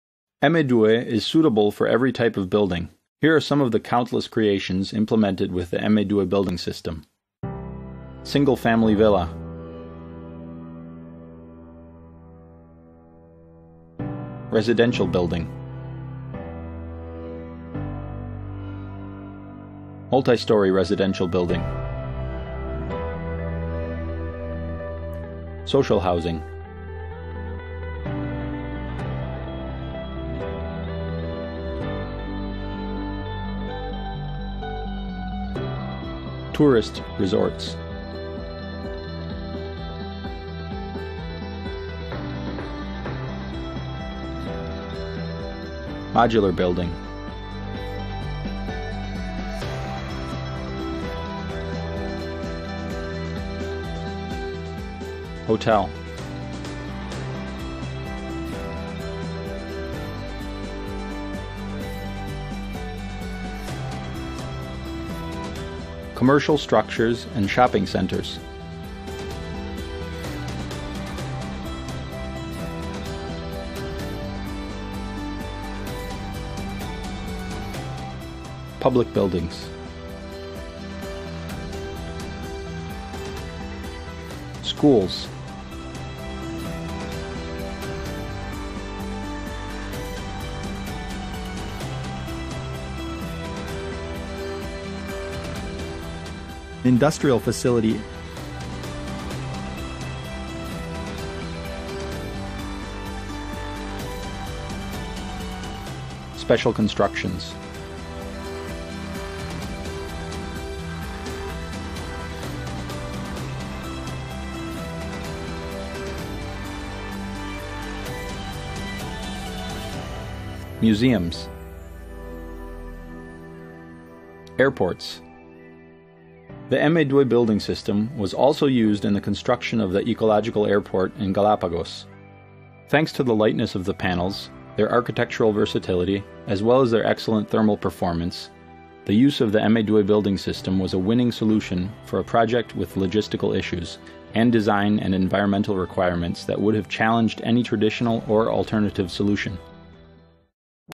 The versatility of the Emmedue Advanced Building System
The Emmedue Advanced Building System promotes complete design flexibility, being equipped with a complete range of building elements: load-bearing walls, curtain walls, floors and stairs. Therefore, the System is suitable for constructing buildings of any type; in addition, it is possible to easily obtain any geometric shape -flat or curved- by simple cutting on-site.